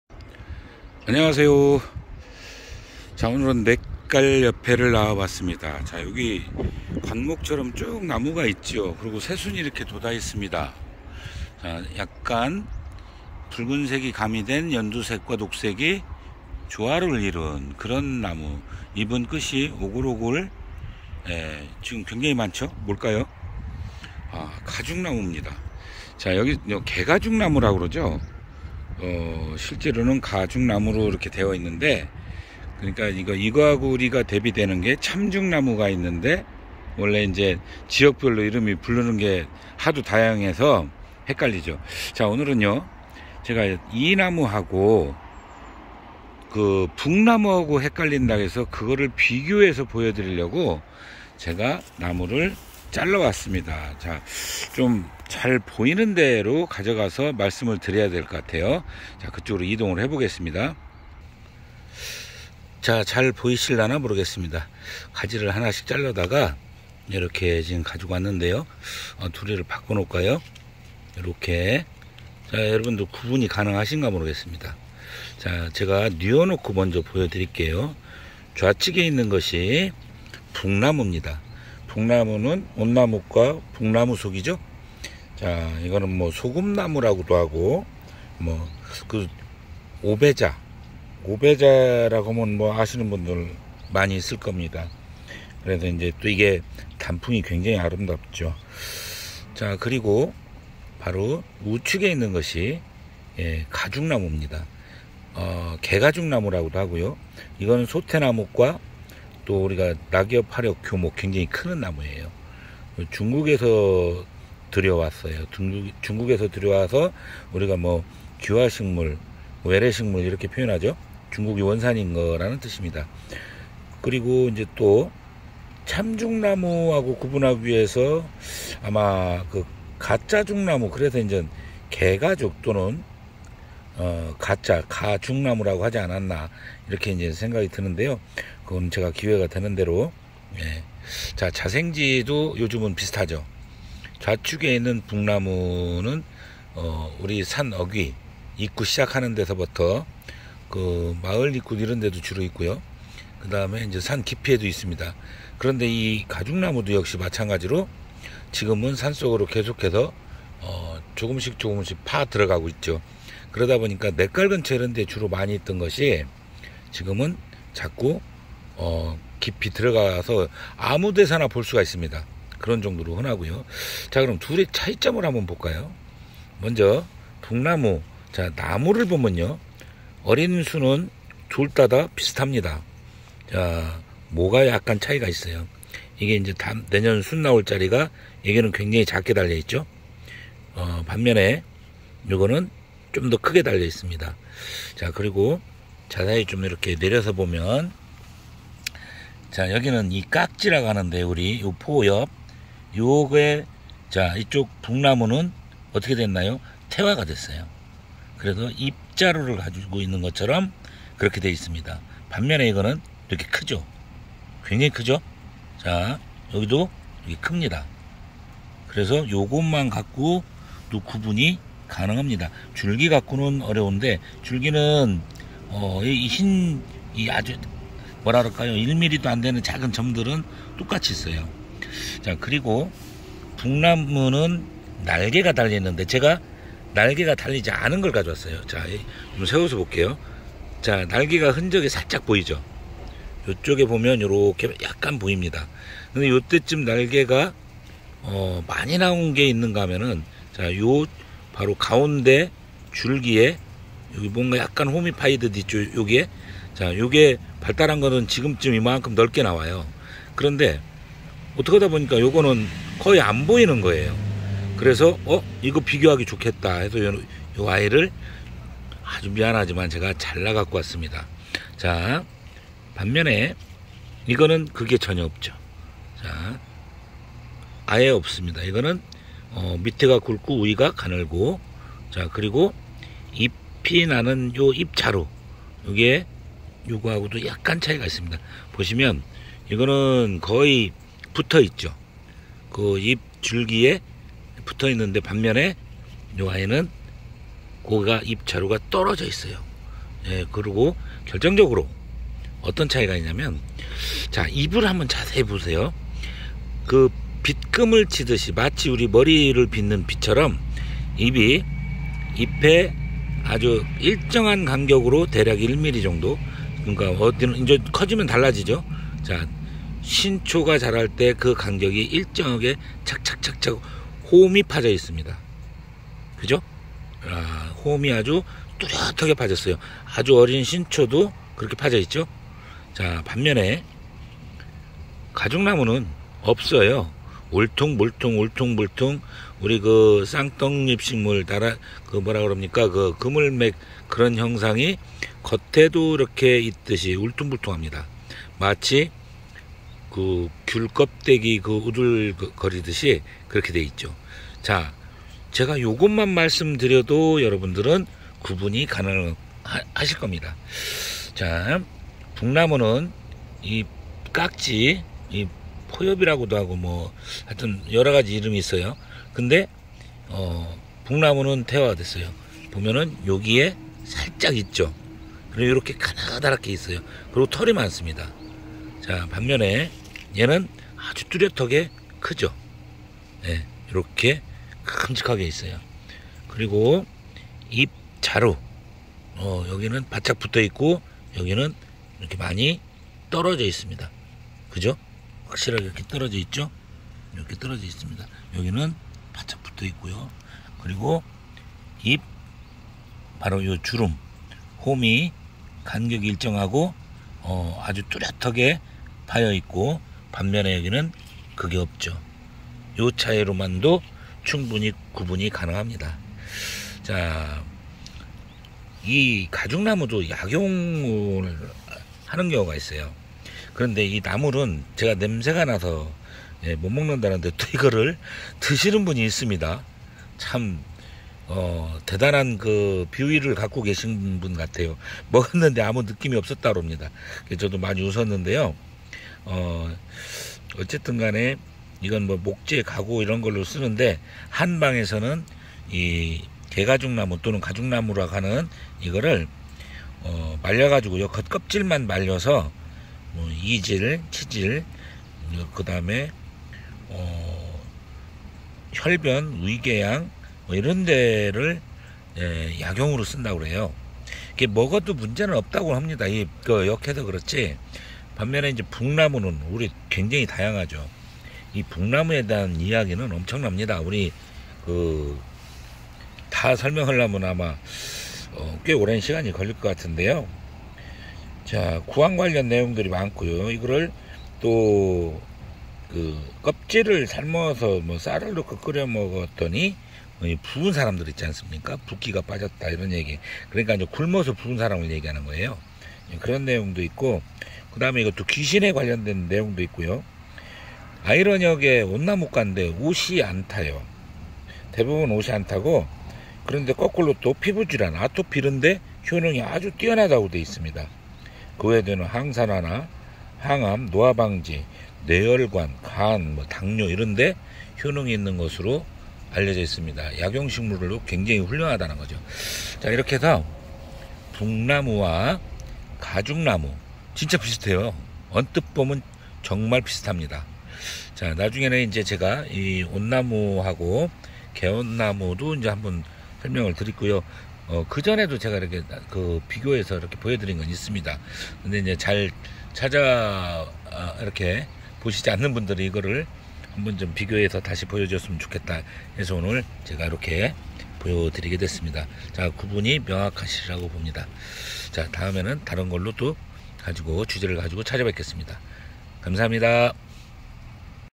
980편, 비슷한 식물 개가죽나무와 붉나무 구분하기
봄에 새순이 나오면 정말 비슷한 식물이 많습니다. 이번에는 개가죽나무와 붉나무 비교 하면서 구분하는 방법을 말씀드립니다.